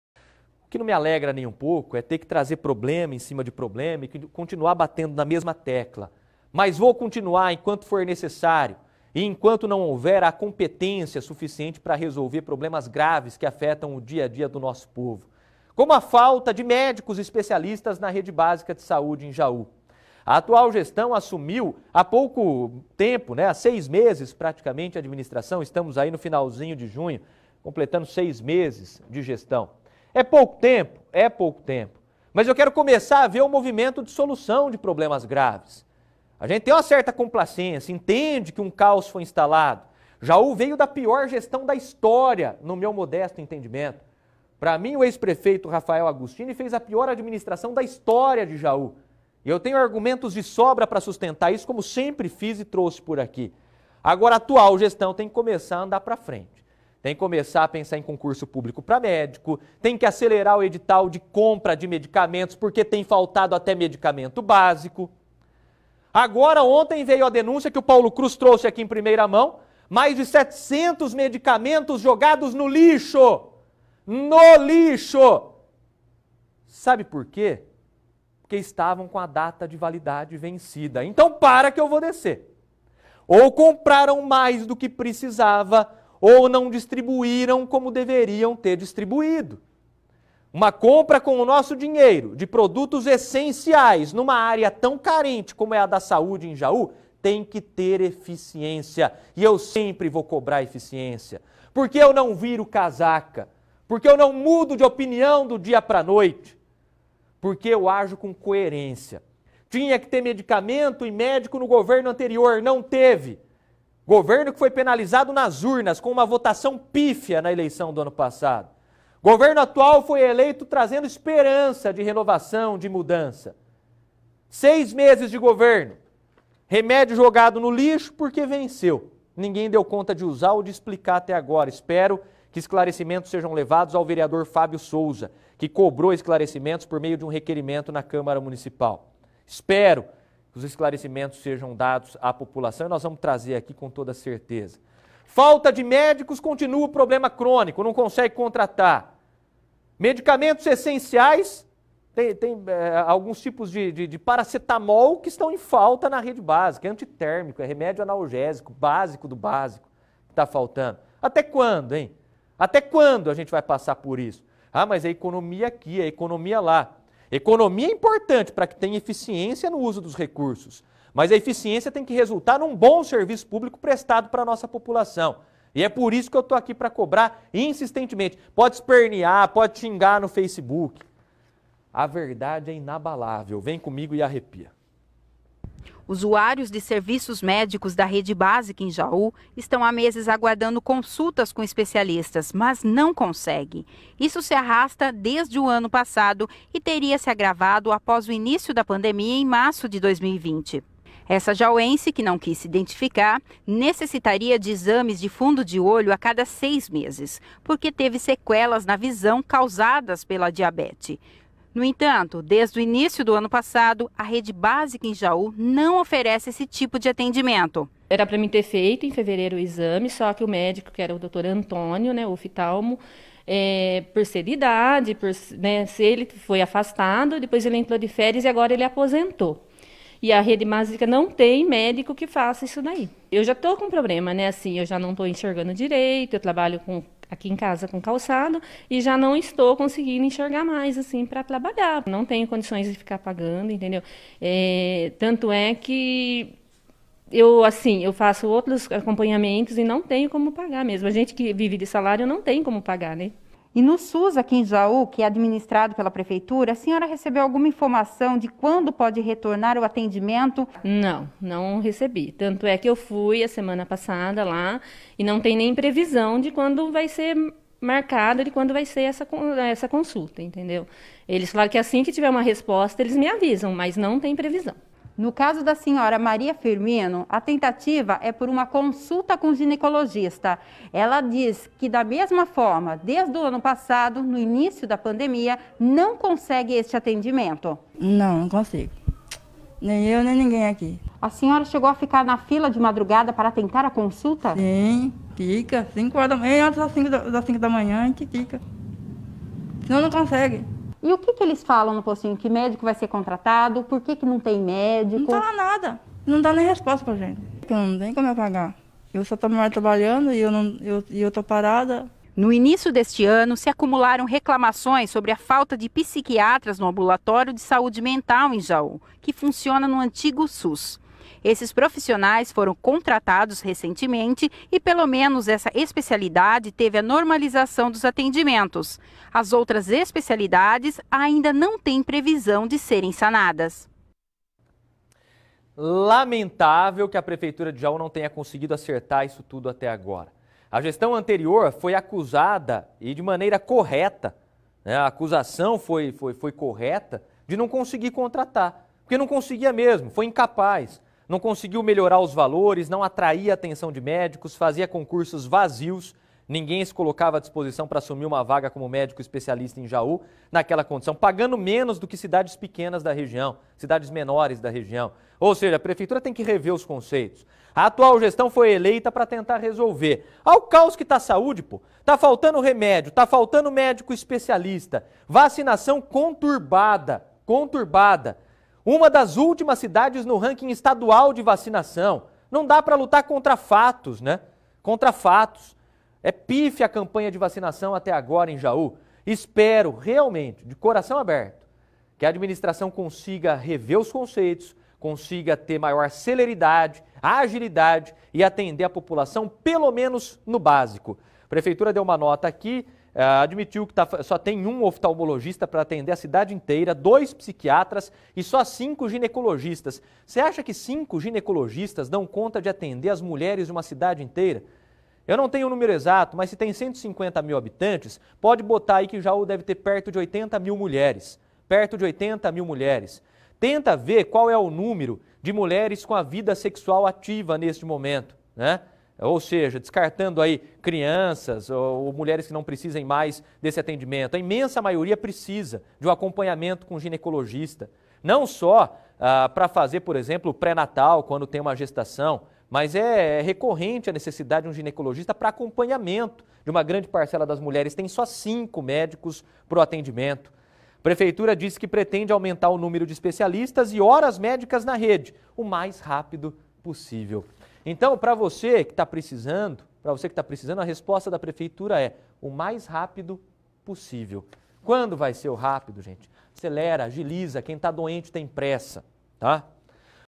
Jaú tem apenas 1 oftalmologista e 2 psiquiatras | Noticidade 22/06/2021
Faltam especialistas na rede básica de saúde.